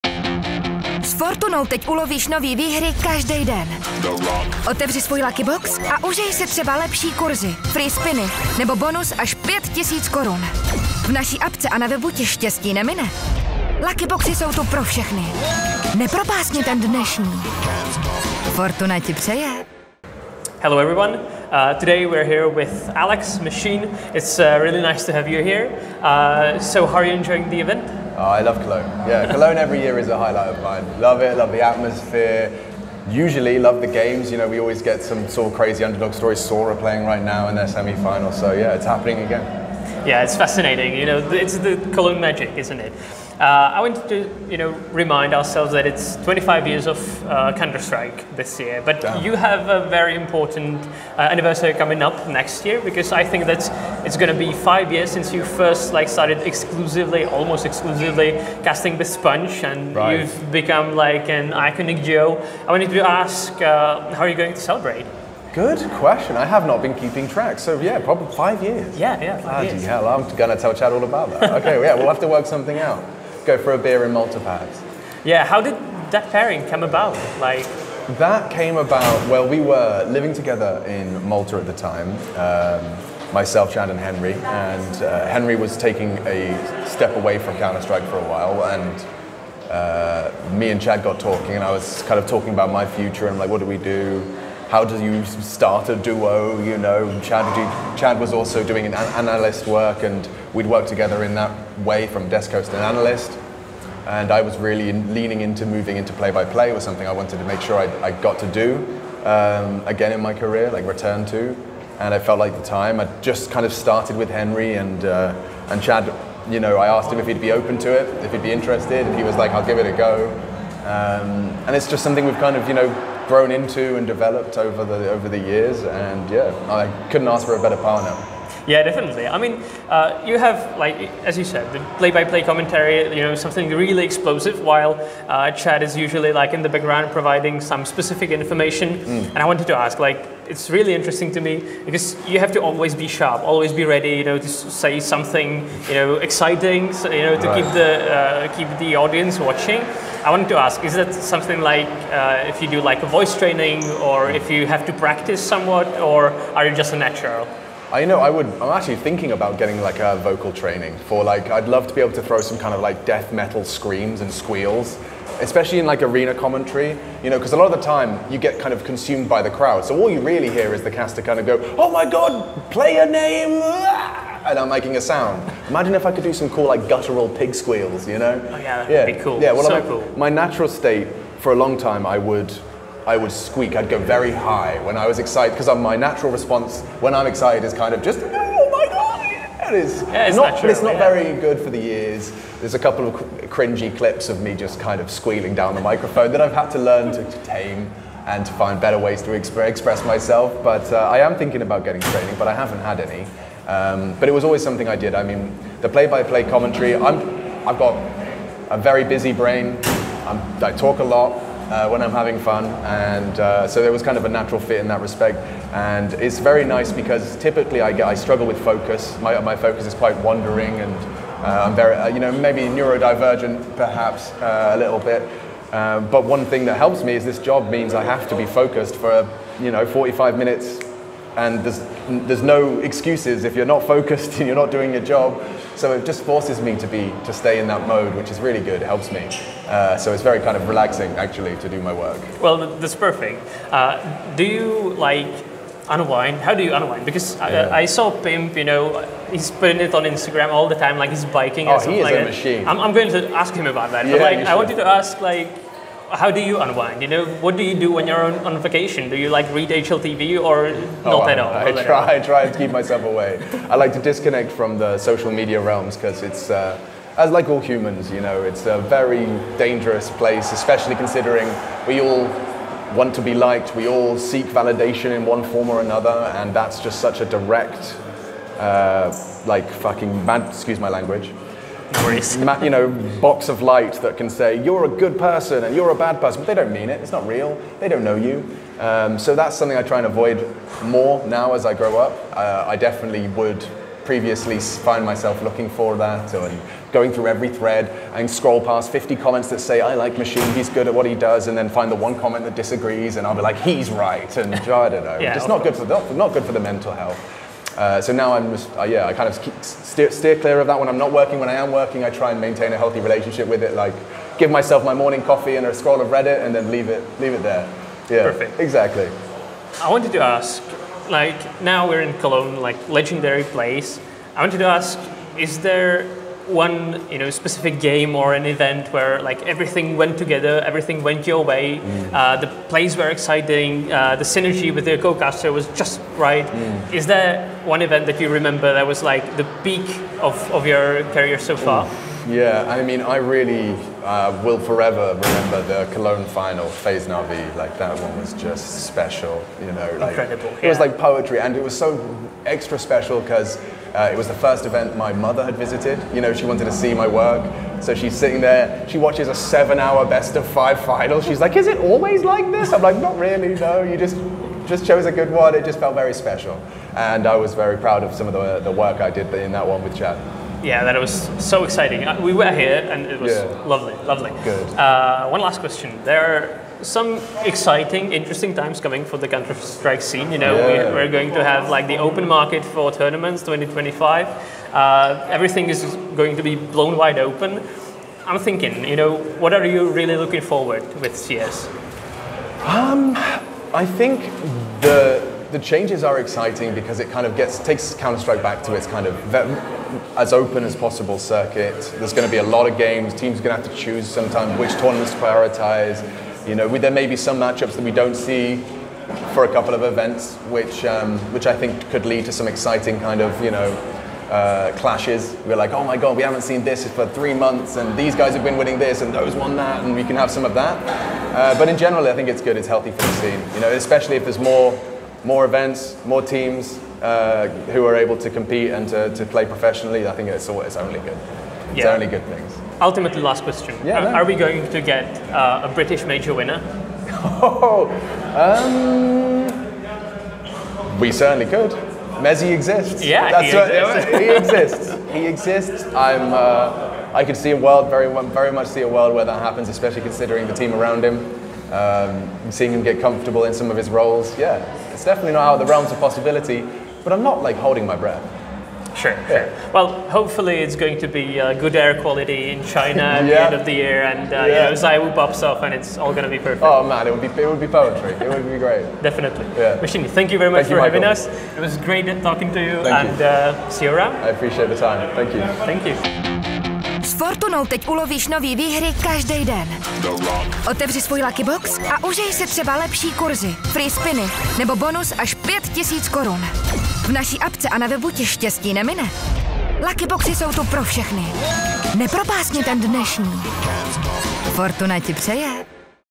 Nemohl bych si přát lepšího parťáka," prozradil Machine o SPUNJovi
V exkluzivním rozhovoru jsme se od Alexe "Machine" Richardsona dozvěděli nejednu informaci o přátelství se SPUNJem a dalších aspektech života komentátora. Co vše jsme z něj dostali?